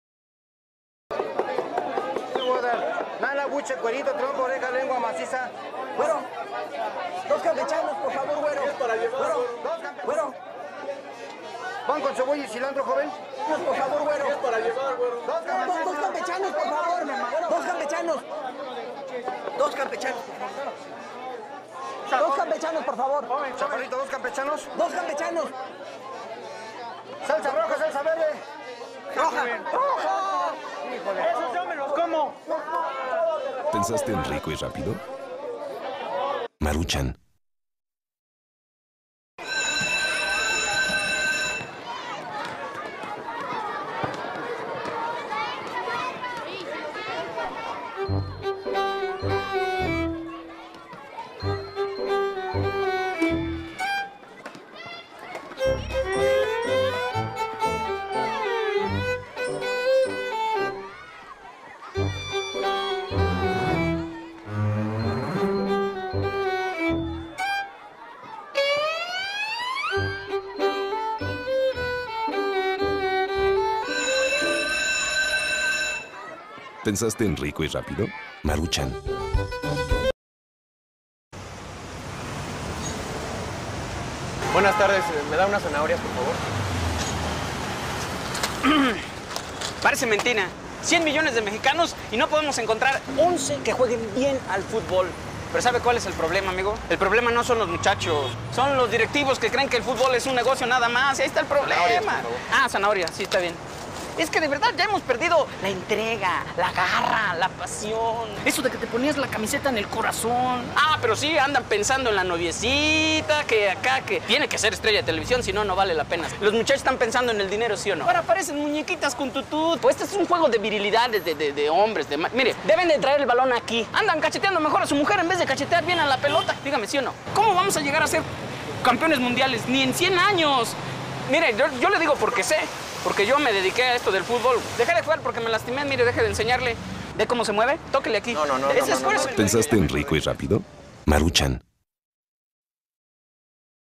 Todos los Comerciales de 2010: Sopa Maruchan
Recopilación de los Comerciales de Sopa Maruchan del Año 2010

Pensante en Rico y Rápido, Maruchan

-Maruchan "Taquero"
-Maruchan "Escuela"
-Maruchan "Fruteo"